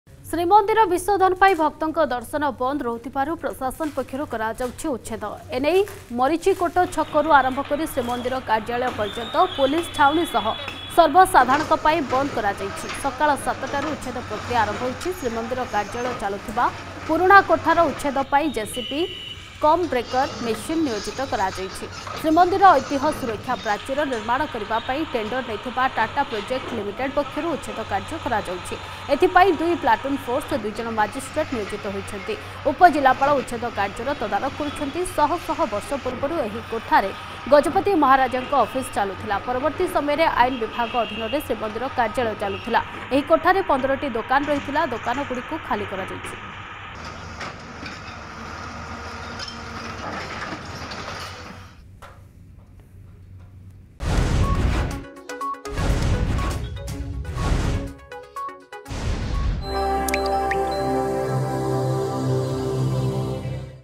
ଶ୍ରୀମନ୍ଦିର ଚାରିକଡରେ ପ୍ରଶାସନ ପକ୍ଷରୁ କରାଯାଉଛି ଉଚ୍ଛେଦ ପ୍ରକ୍ରିୟା
ଶ୍ରୀମନ୍ଦିର ବିଶୋଧନ ପାଇଁ ଭକ୍ତଙ୍କ ଦର୍ଶନ ବନ୍ଦ ରହୁଥିବାରୁ ପ୍ରଶାସନ ପକ୍ଷରୁ କରାଯାଉଛି ଉଚ୍ଛେଦ । ଏ ନେଇ ମରଚିକୋଟ ଛକରୁ ଆରମ୍ଭ କରି ଶ୍ରୀମନ୍ଦିର କାର୍ଯ୍ୟାଳୟ ପର୍ଯ୍ୟନ୍ତ ପୋଲିସ ଛାଉଣି ସହ ସର୍ବସାଧାରଣଙ୍କ ପାଇଁ ବନ୍ଦ କରାଯାଇଛି ।